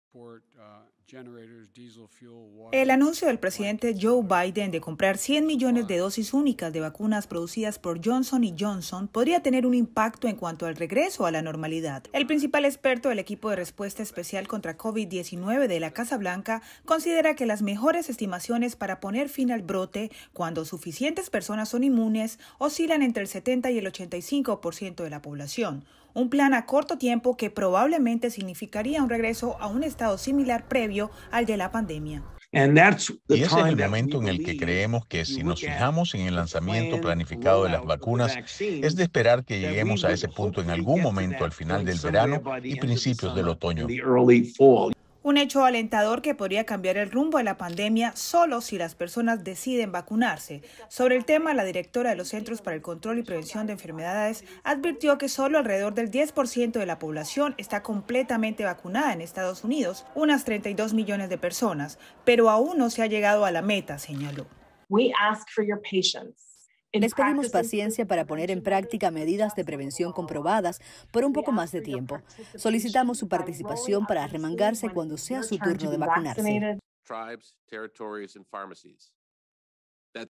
Biden confirma la compra de 100 millones más de vacunas
El presidente Joe Biden confirmó hoy la compra de más vacunas contra el COVID-19, como una medida para inmunizar a los estadounidenses en el menor tiempo posible. Divalizeth Cash tiene todos los detalles.

¡Visítenos!